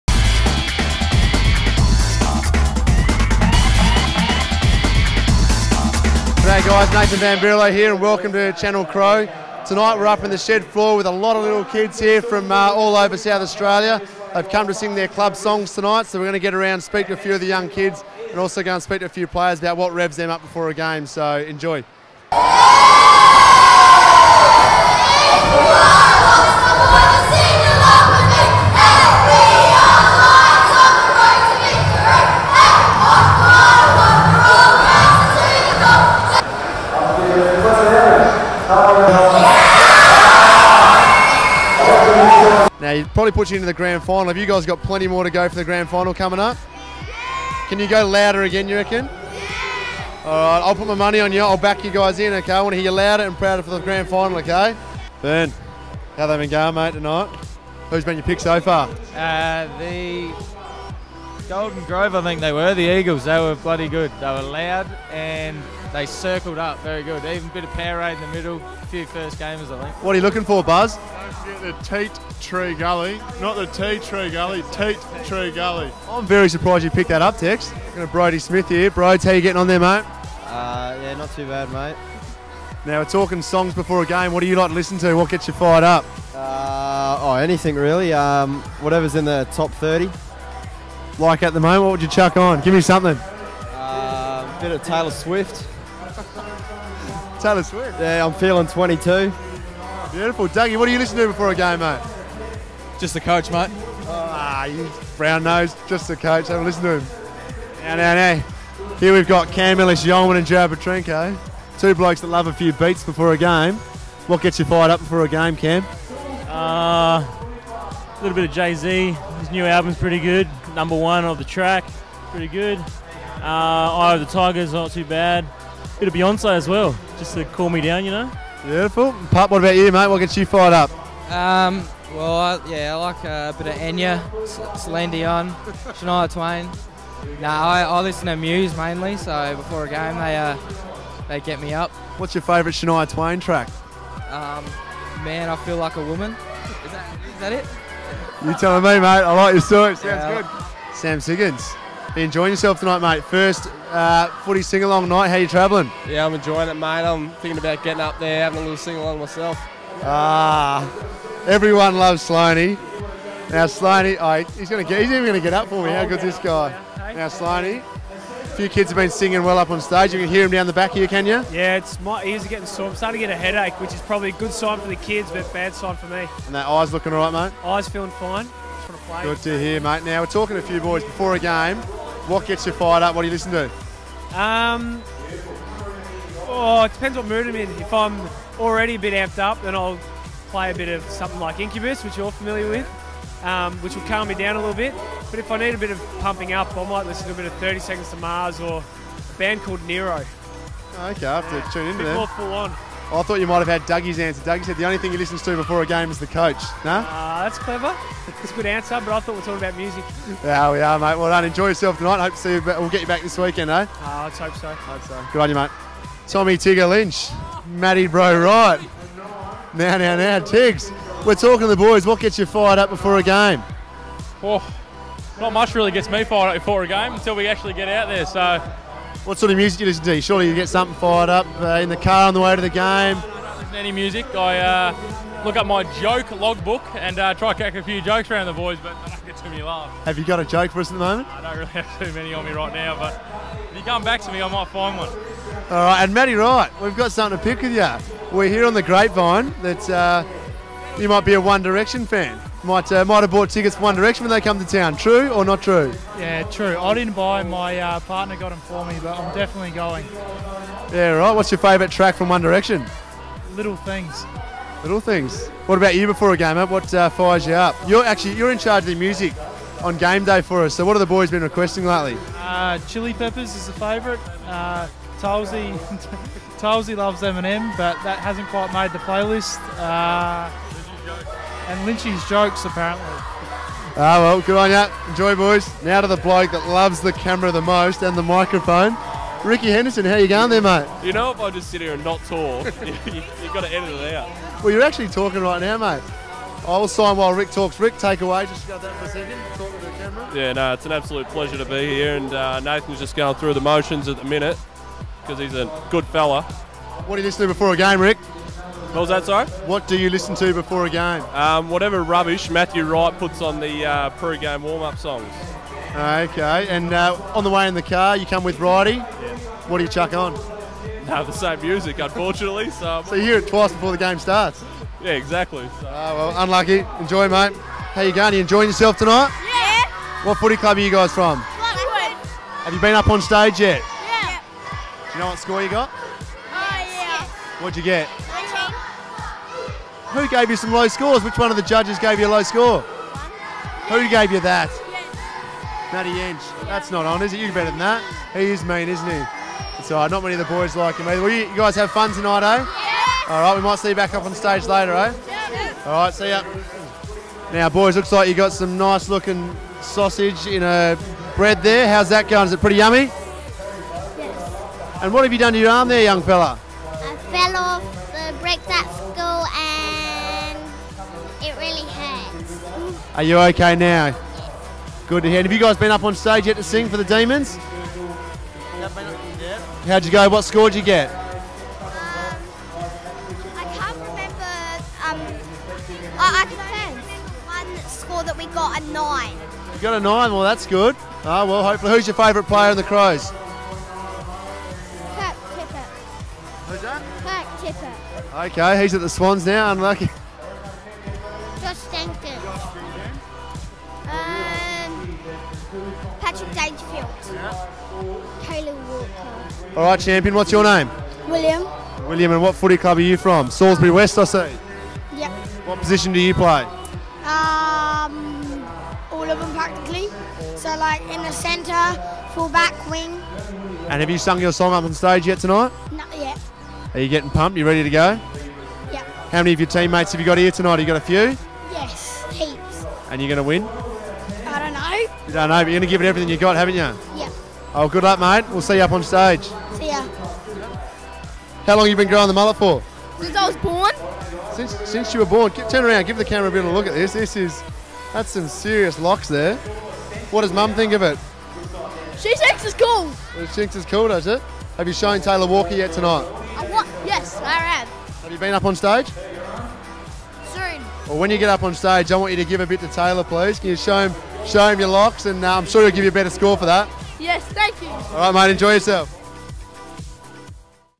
VB at Crows in Clubs Sing Off
Adelaide Captain Nathan van Berlo took the mic for Channel Crow at the annual Crows in Clubs sing off night!